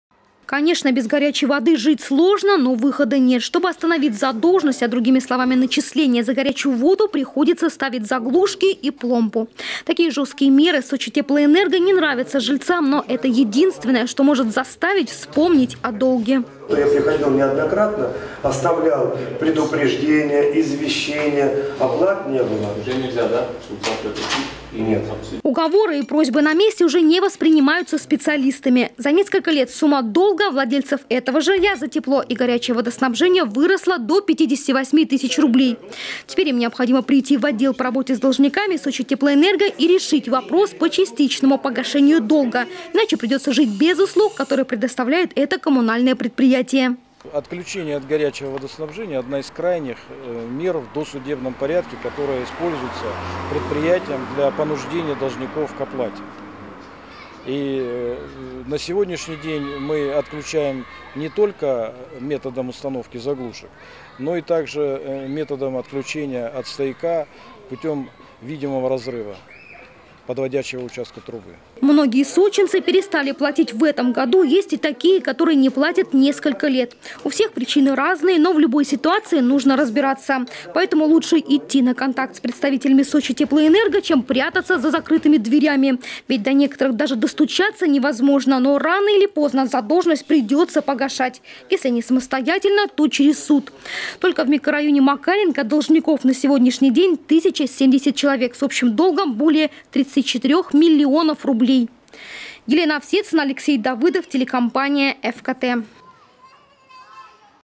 «Сочитеплоэнерго» продолжает отключать должников от горячей воды. Новости Эфкате Сочи
К таким вынужденным мерам приводят большие долги населения. У большинства должников сумма задолженности свыше 50 тысяч рублей.

Конечно, без горячей воды жить сложно, но выхода нет. Чтобы остановить задолженность, а другими словами начисления за горячую воду — приходится ставить заглушки и пломбу. Такие жёсткие меры «Сочитеплоэнерго» не нравятся жильцам, но это единственное, что может их заставить вспомнить о долге.

Уговоры и просьбы на месте уже не воспринимаются специалистами. За несколько лет сумма долга владельцев этого жилья за тепло и горячее водоснабжение выросла до 58 тысяч. Теперь им необходимо прийти в отдел по работе с должниками «Сочитеплоэнерго» и решить вопрос по частичному погашению долга. Иначе придётся жить без услуг, которые предоставляет это коммунальное предприятие.

Сочи. Новости.
Телеканал Эфкате.